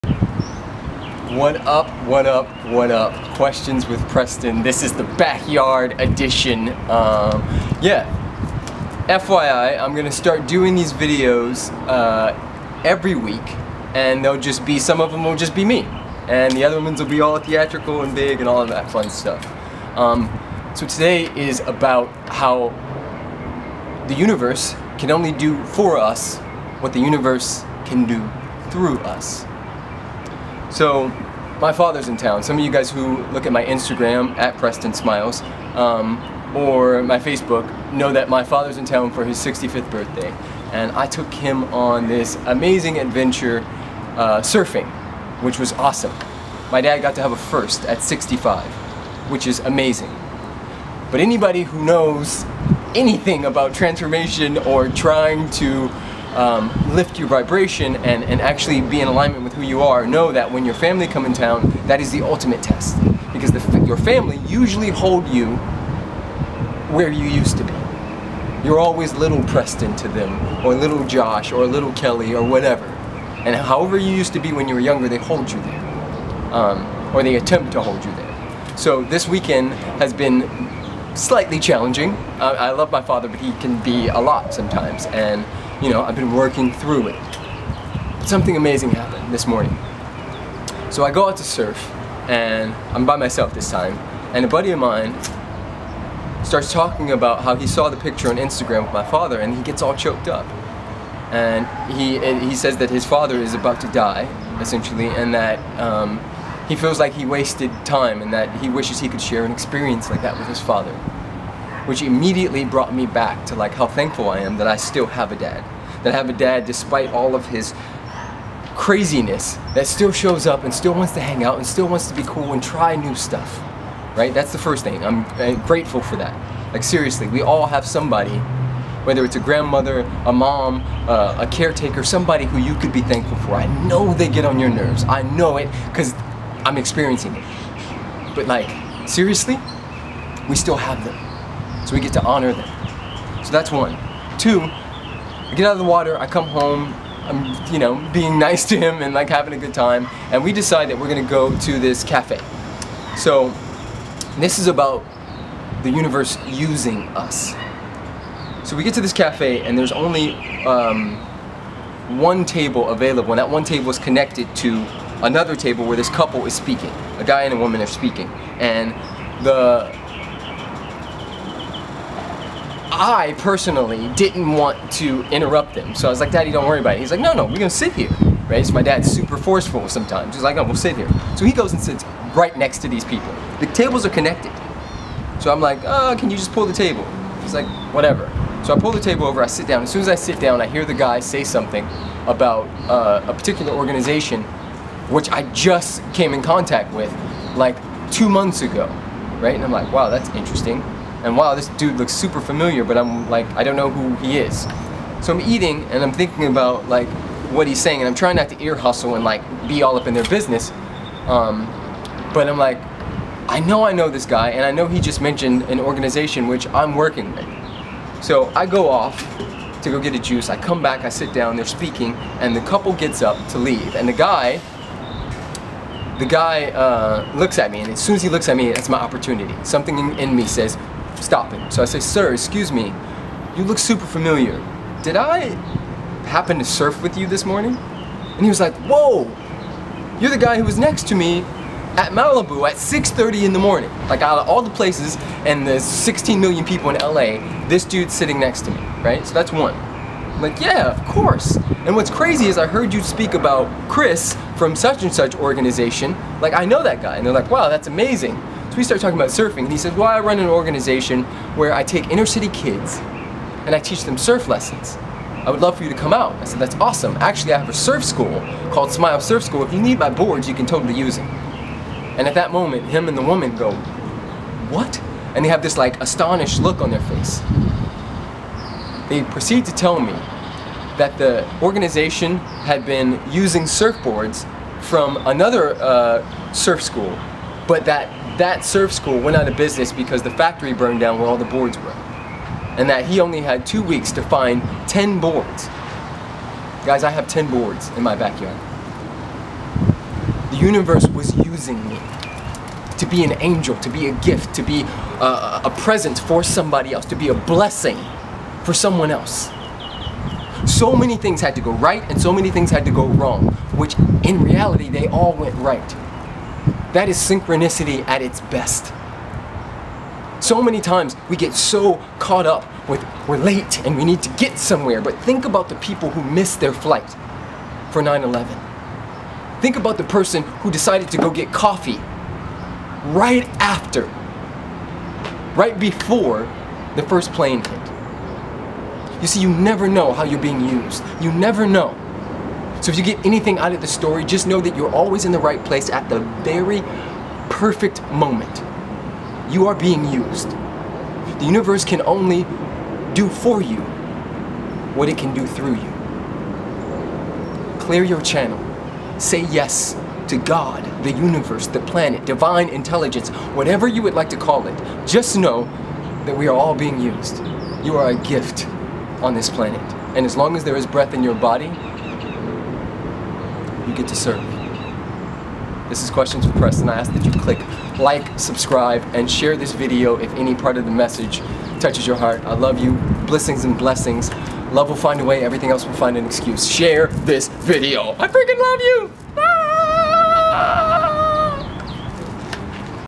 BE THE CHANNEL FOR GOOD -- Synchronicity
Be grateful for everyone in your life. We are all being used to help each other in service of a Higher Power. Synchronicity. You are exactly where you’re supposed to be in every moment.